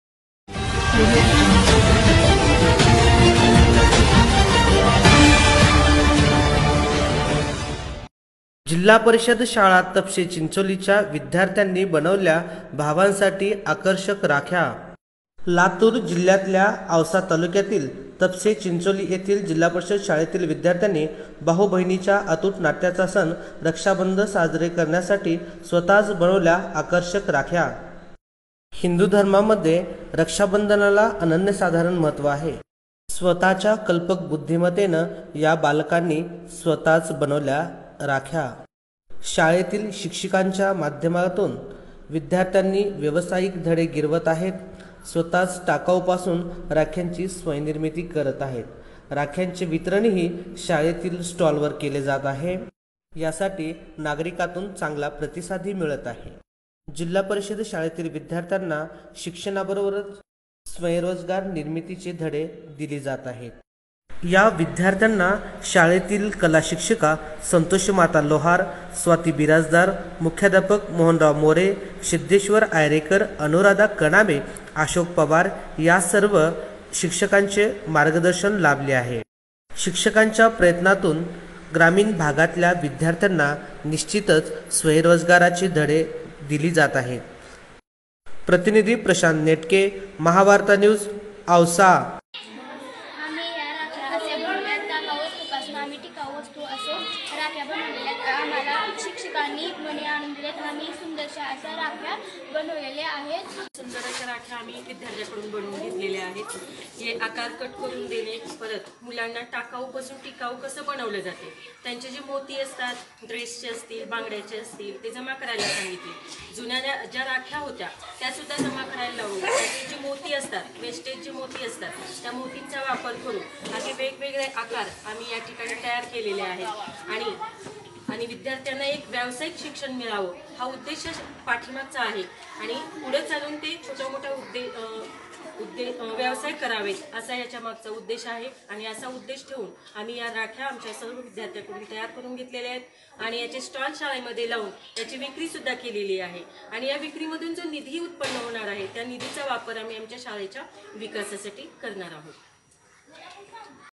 तपसे चिंचोली शाळेच्या विध्यार्थ्यांचा अनोखा व्यवसायिक उपक्रम...रक्षाबंधन स्पेशल.
तपसे चिंचोली जिल्हा परिषद शाळेचा अनोखा व्यावसायिक उपक्रम

औसा :- औसा तालुक्यातील तपसे चिंचोली  जिल्हा परिषद शाळेतील विद्यार्थ्यांनी भाऊबहिणी च्या अतूट नात्याचा सण अनोखे रक्षाबंधन साजरे करण्याच्या हेतूने  शाळेत आकर्षक राख्या बनवल्या आहेत.घरगुती पध्दतीने बनवलेल्या या राख्याने चिंचोलीकरांचा  रक्षाबंधन हा सण अधिकच आकर्षक ठरणार आहे.
 बाजारात विक्रीसाठी उपलब्ध असणाऱ्या राख्या सहज आपल्या गावात उपलब्ध व्हाव्या आणि तसेच  विद्यार्थ्यांना आतापासूनच उद्योग क्षेत्रात भविष्यात अग्रेसर राहण्याची गरज आहे हे ओळखून  तपसे चिंचोली शाळेतील शिक्षिका  संतोषीमाता लोहार यांच्या प्रमुख भूमिकेत शाळेतील विद्यार्थ्यांना कार्यानुभव विषयानिमित्ताने  आकर्षक राख्या बनवण्याचा उपक्रम राबविण्यात आला .शाळेतील शिक्षिका  संतोषीमाता लोहार मॅडम व सेविका स्वाती बिराजदार यांनी आकर्षक राख्याची माहिती व प्राथमिक साहित्य उपलब्ध करून विद्यार्थ्यांच्या सहकार्याने राख्या बनवल्या आहेत.शाळेतील 5 वी ते  7 वी वर्गातील विद्यार्थी या उपक्रमात मोठया हर्ष उल्हासाने सहभागी  होऊन  सुंदर राख्या बनवण्यात मग्न झाले होते. राख्यासाठी लागणारे साहित्य आणण्यापासून ते राख्या बनवून राख्याला आकर्षकपणा  देऊन  सुंदर बनवण्यासाठी शाळेच्या शिक्षिका संतोषीमाता लोहार मॅडम  यांच्या उपस्थितीत विद्यार्थ्यांकडून करून घेऊन हा उपक्रम घेण्यात आला.
या राख्याचे वितरण विक्री शाळेत स्टॉल उभे करून विद्यार्थ्यांमार्फ़त विक्री केली करून जमा झालेला निधी  विद्यार्थ्यांच्या सर्वांगीण विकासासाठी वापरला जाणार असल्याची माहिती शाळेतील शिक्षकांच्या सहमतीने देण्यात आली आहे.
तरी गावातील सर्वच लोकांनी या विद्यार्थ्यांनी आपल्या सुंदर हातांनी बनवल्या या राख्या घेऊनच रक्षाबंधन सण साजरा करावा असे आवाहन शाळेमार्फत करण्यात आले आहे.जिल्हा परिषद शाळेतील विद्यार्थी सुद्धा आकर्षक  व्यावसायिक उपक्रम राबविण्यात अग्रेसर आहेत हे  चिंचोली शाळेतील विद्यार्थ्यांनी करून दाखवून दिले आहे.
या उपक्रमातून ग्रामीण भागातील विद्यार्थ्यांना रोजगाराचे ज्ञान आतापासूनच मिळत आहे.
त्यांच्या या उपक्रमाला साथ हवी आहे ती आपल्या सर्वांच्या सहकार्याची.या उपक्रमाच्या यशस्वीते साठी शाळेतील मुख्याध्यापक मोहनराव मोरे, शिक्षक  सिद्धेश्वर आयरेकर,देवानंद कोंडमगिरे, प्रदीप इज्जपवार, धर्मराज भिसे, अशोक पवार,अनुराधा कनामे (मॅडम) यांनी विद्यार्थ्यांना मार्गदर्शन केले आहे.